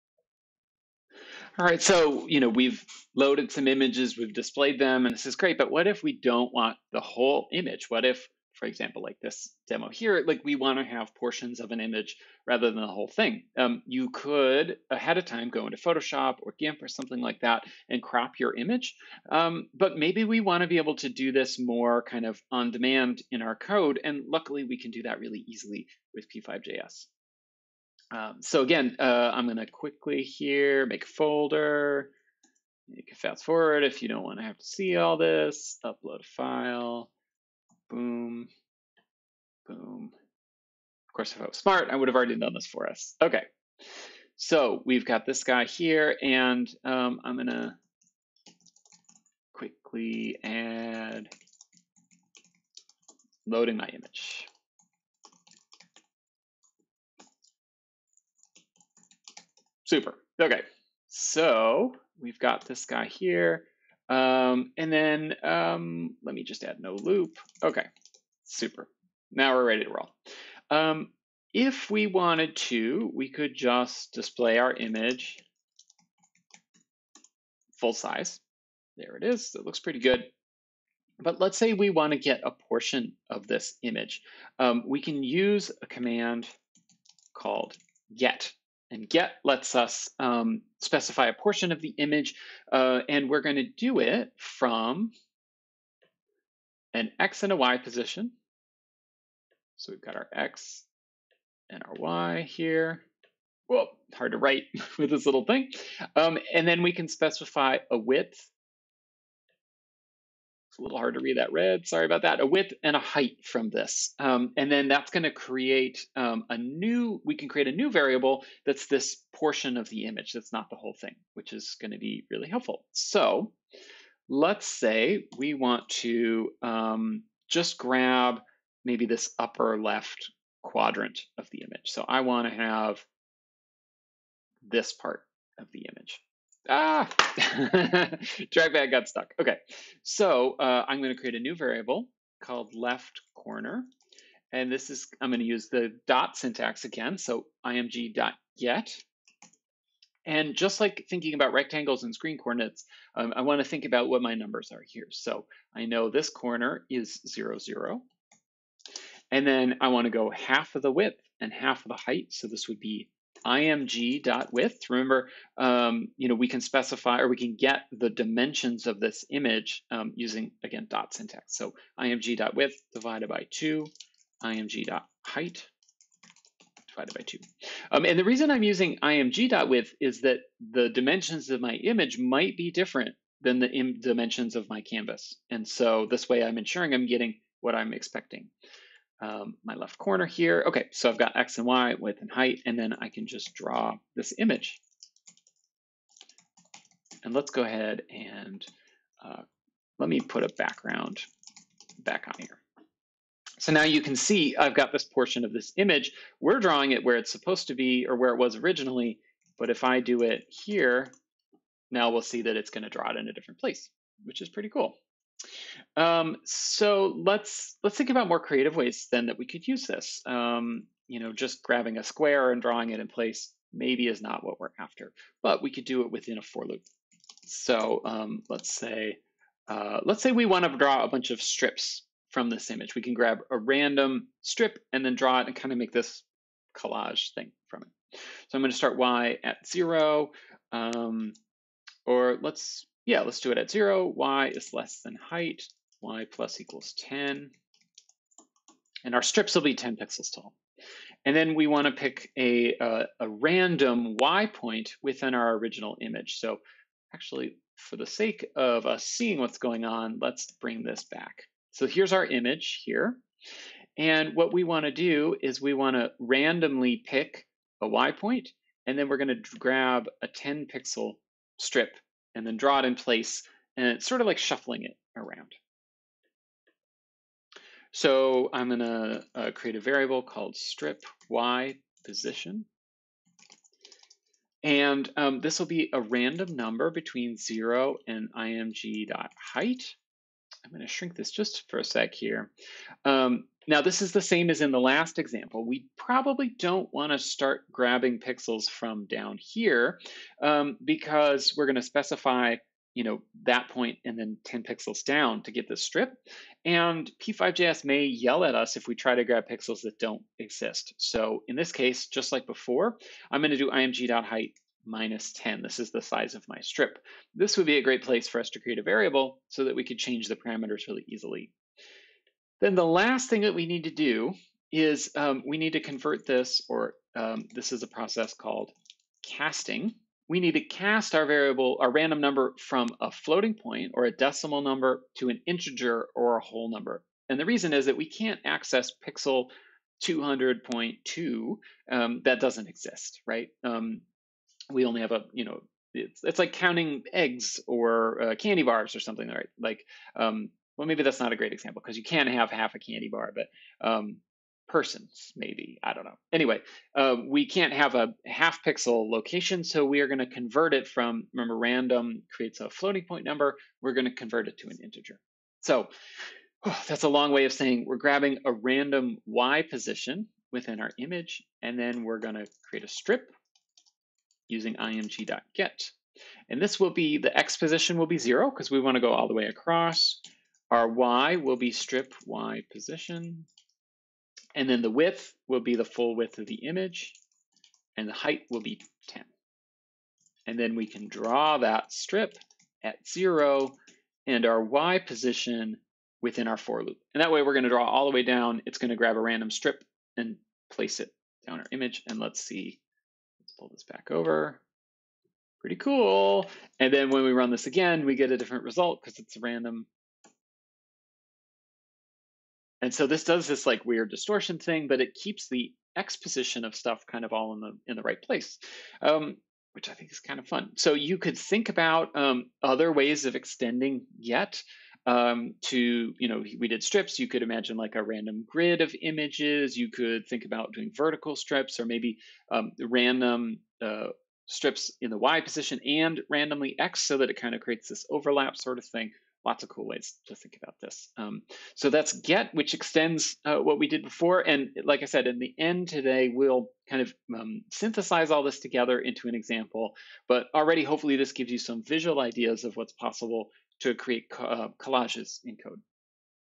CP1: Collage in P5JS – Getting Portions Of An Image
With loadImage() and resize(), we can easily load and display images. But if we want to make collages, we'll want a way to cut out a portion of an image. There lots of mathy ways we might think about doing this, but p5.js comes to the rescue with a much easier tool: get()

CHALLENGES
❓ Can you grab random squares from the source image and draw them in random locations? Can you add random rotation as well? (Hint: use push() and imageMode(CENTER) to draw the images from the center like ellipse() does)